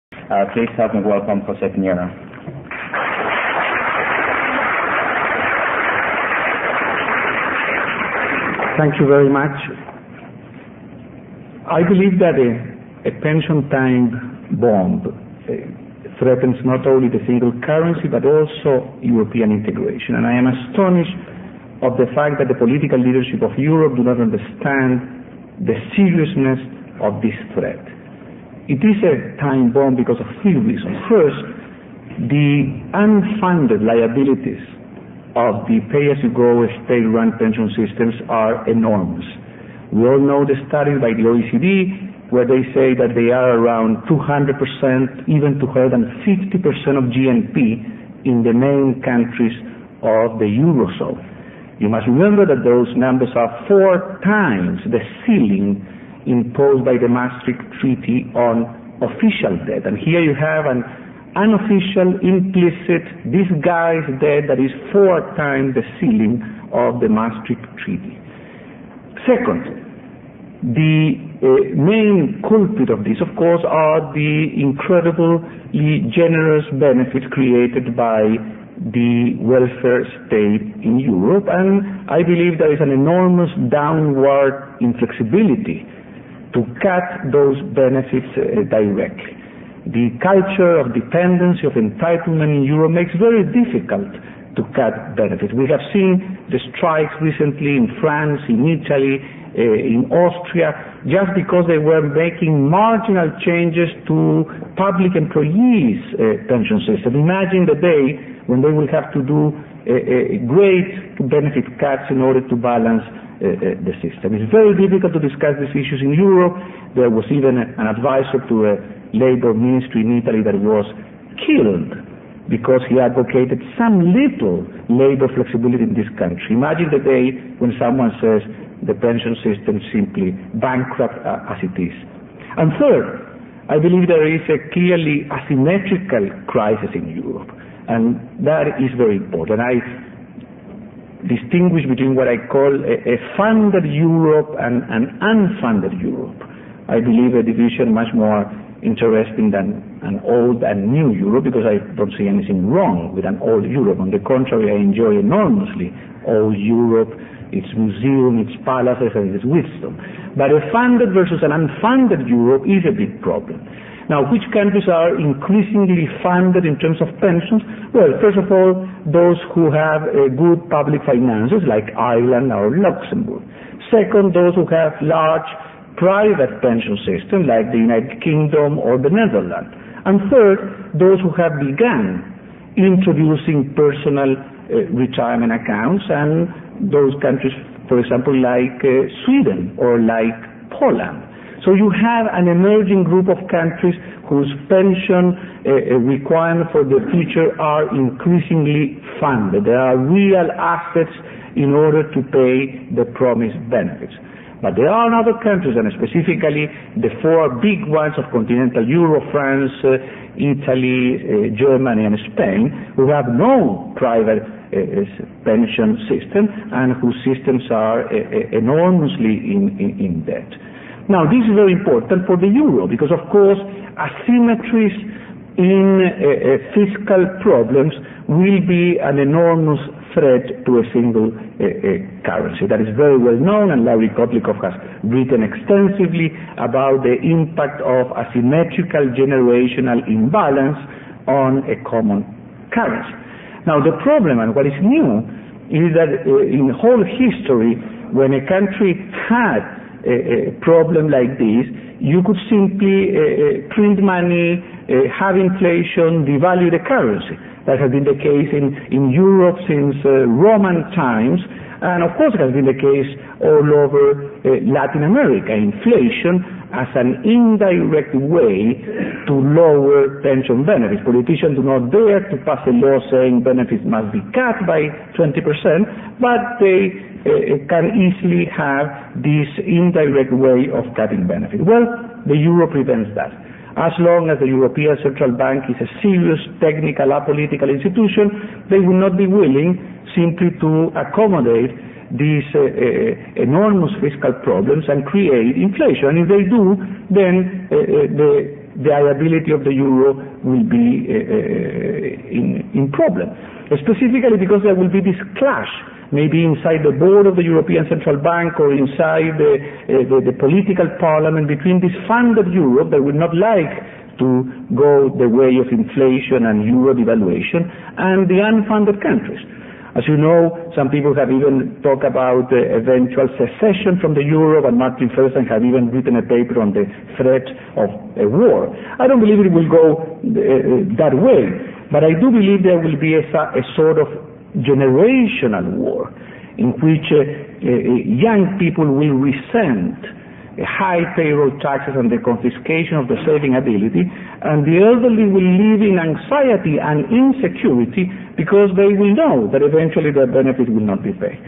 José Piñera - 21st Annual Monetary Conference / Part 1
In 2003 José Piñera, President of the International Center for Pension Reform and Distinguished Senior Fellow of the Cato Institute, gave this conference warning the upcoming crisis of the west. For more information about José Piñera's ideas and Pension Reform